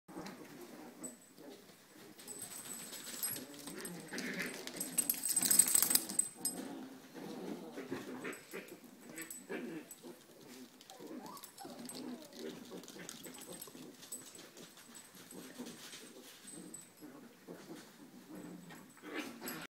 Italian Greyhounds pups
Winnie and the pups playing.